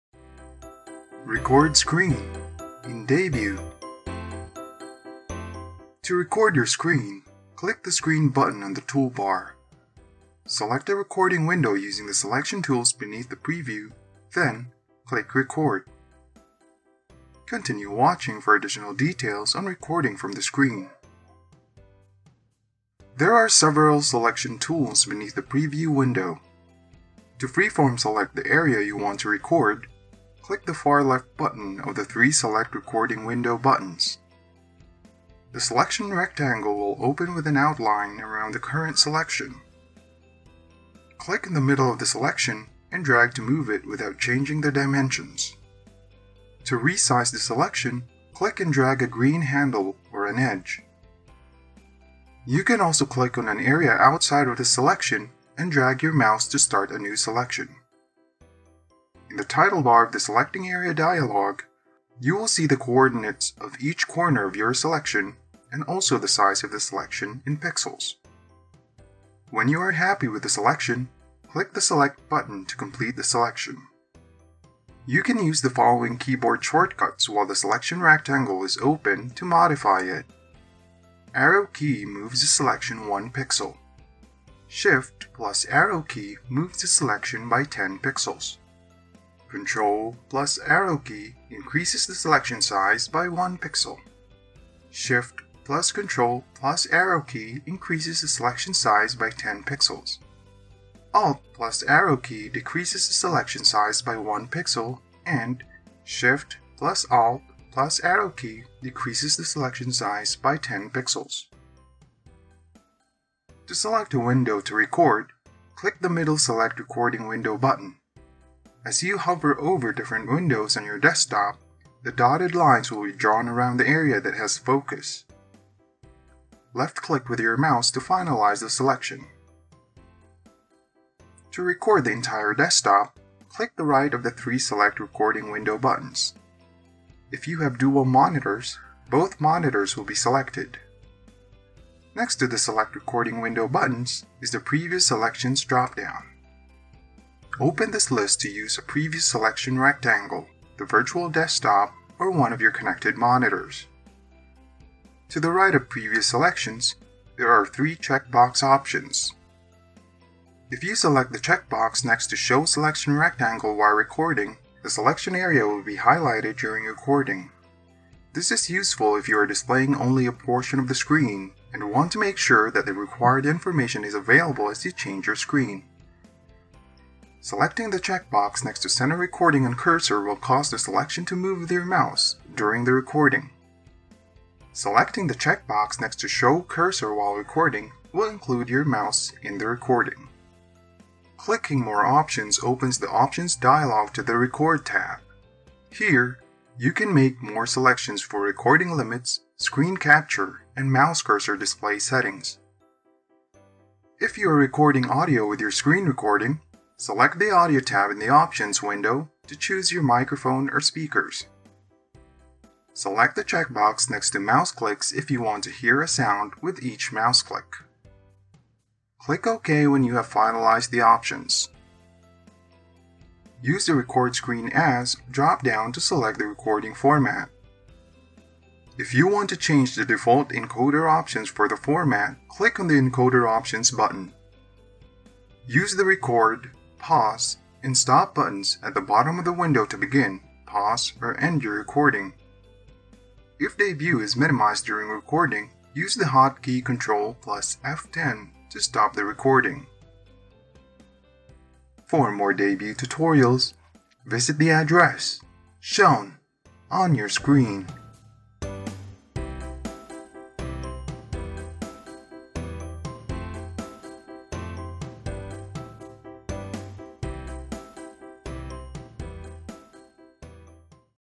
How to Record Screen | Debut Video Capture Software Tutorial
This tutorial video will show you how to record a video of your computer screen with Debut Video Capture Software.

To record your screen, click the Screen button on the toolbar, select a recording window using the selection tools beneath the preview, then, click Record.

Do you need help using Debut?